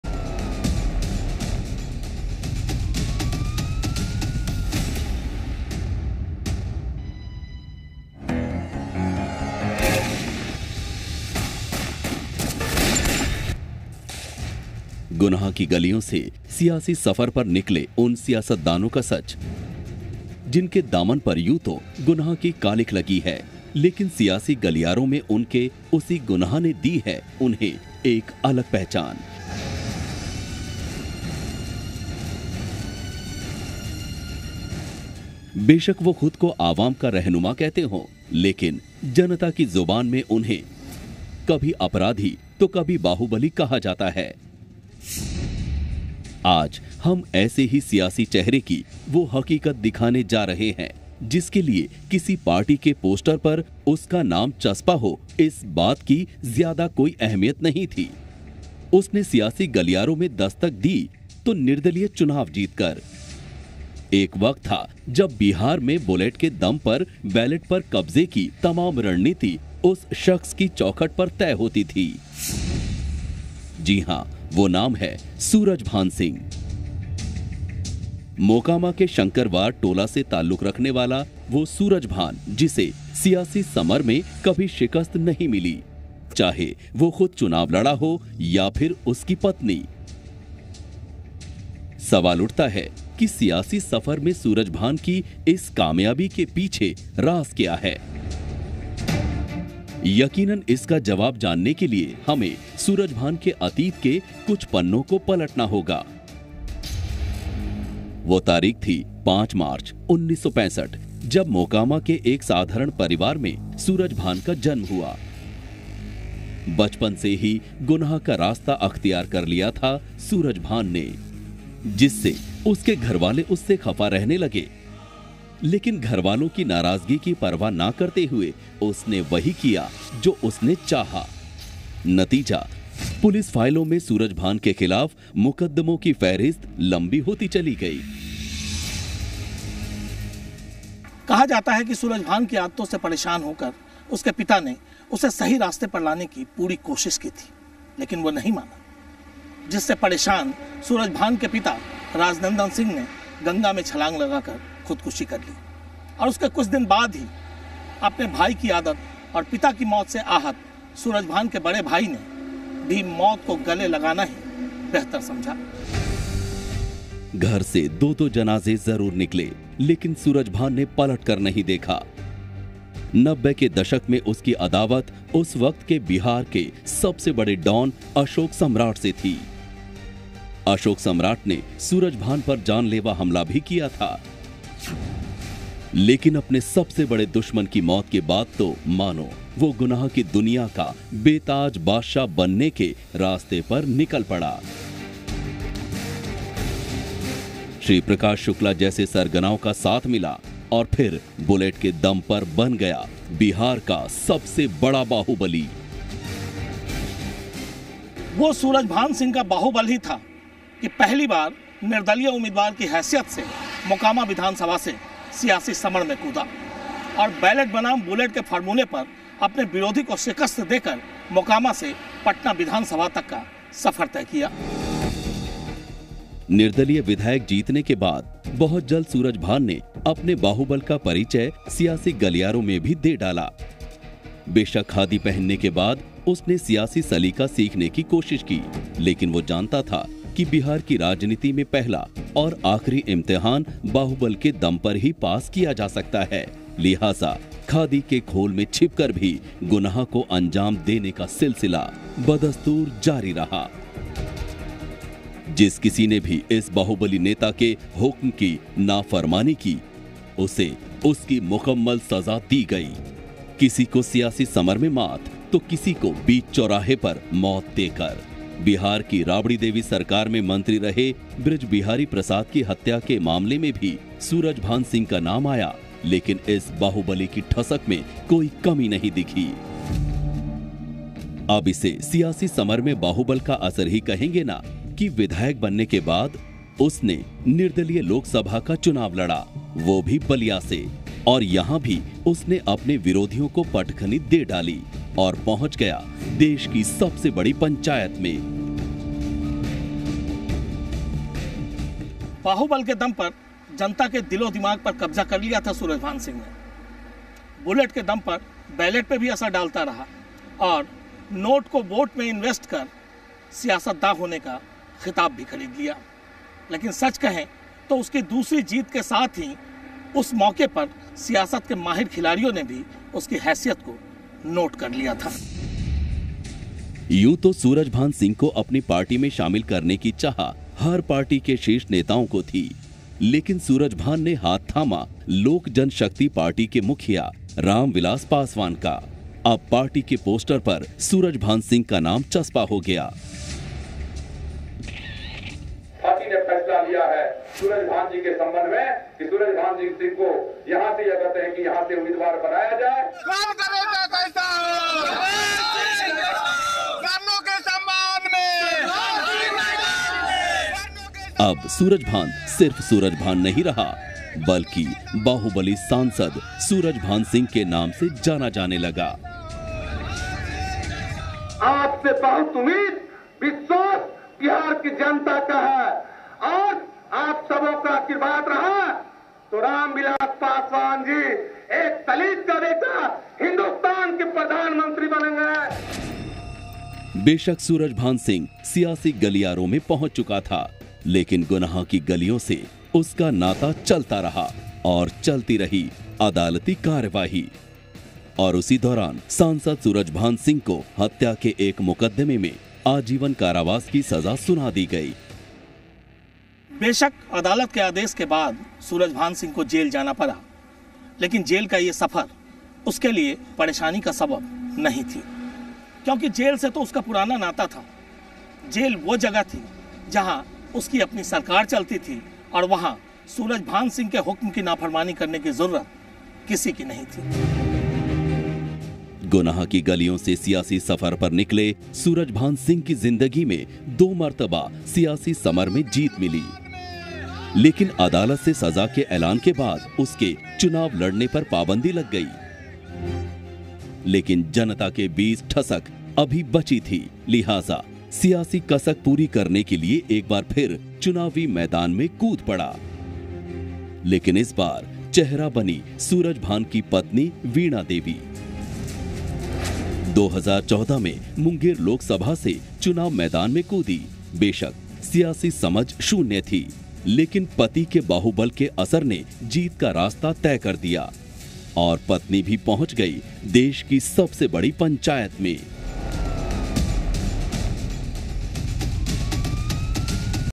SPECIAL: सूरजभान सिंह की अनसुनी कहानी
सूरजभान सिंह का गुनाह की गलियों से सियासत तक का सफर दिलचस्प है.  मोकामा के सूरजभान को सियासी समर में कभी शिकस्त नहीं मिली. सूरजभान सिंह का अतीत गुनाह के रास्तों से होकर गुजरा है.